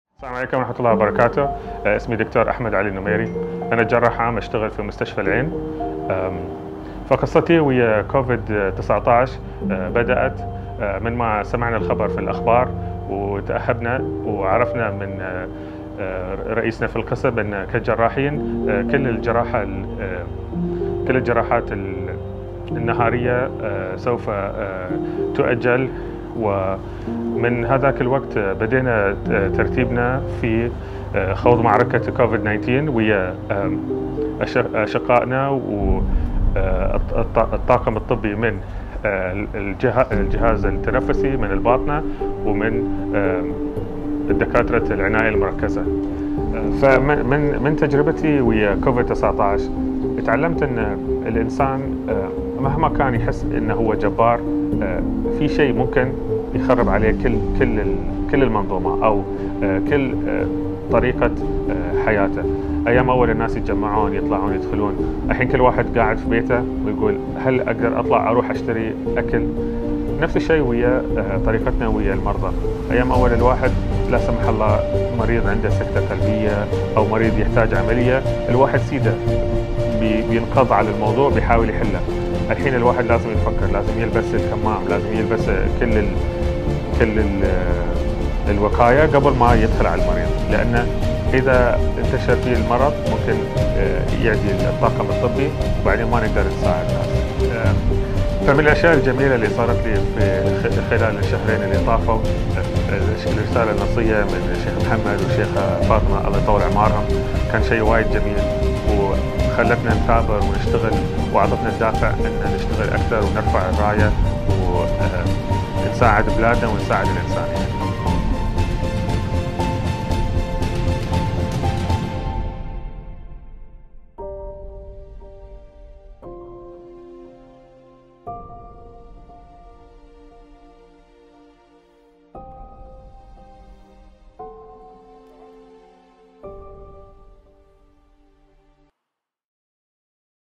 Dr. Ahmed Al-Numairy interview June 2020
Dr. Ahmed Al-Numairy last interview May 2020 sharing his experience fighting the pandemic as a surgeon working with ICU patients